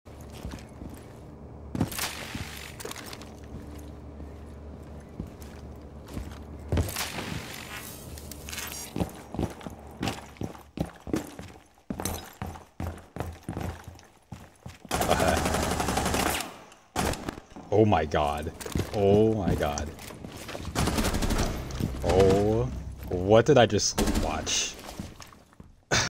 The most free kill ever
This guy whiffed on my clone and I just knew I was getting a kill.